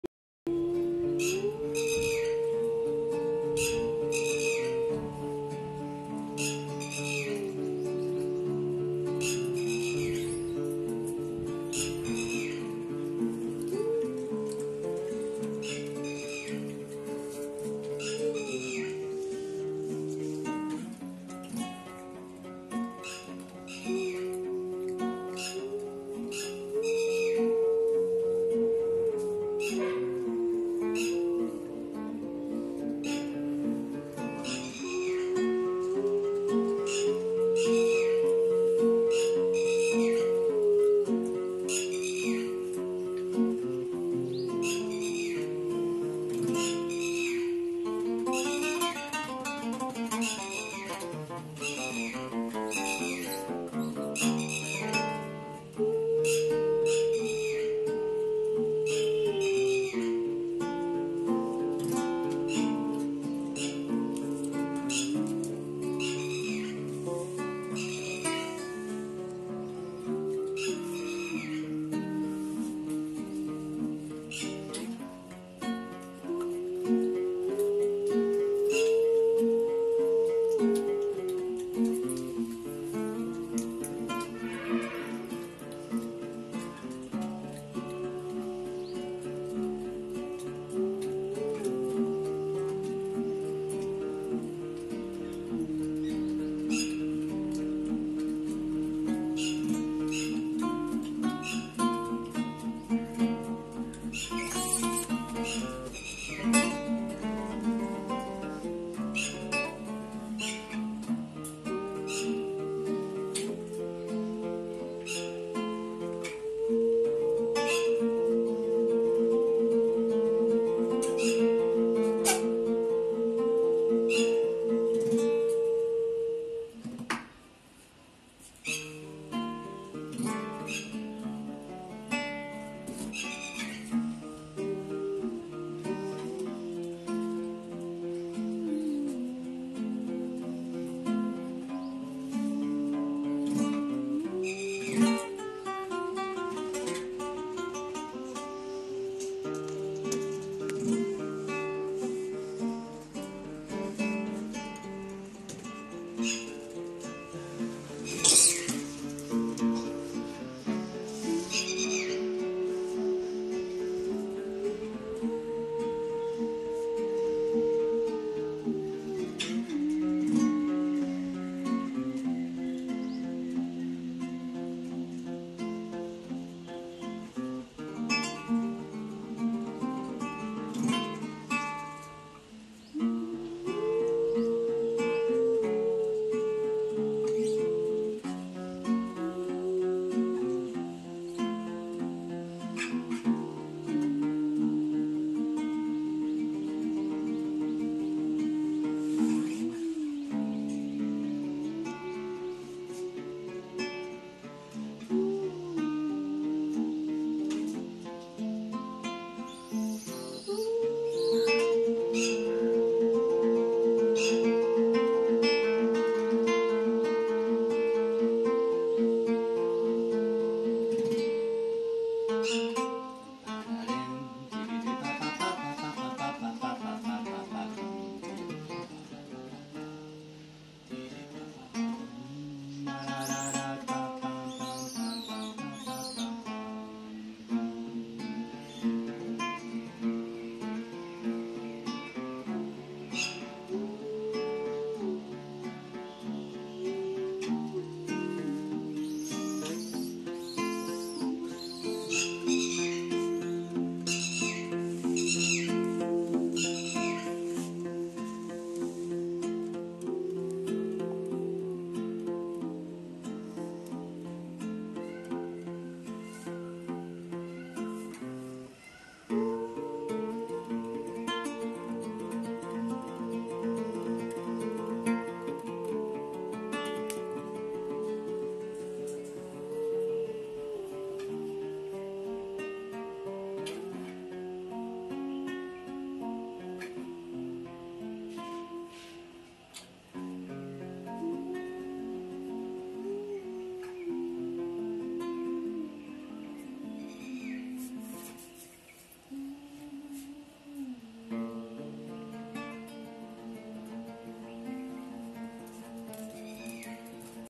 Monkeys love music. Guitar for langurs. Animals and music. Pau Saura.
Improvisation with friendly langurs. Beings of love. Loving animals. Recorded in Pushkar (India) in May 2020.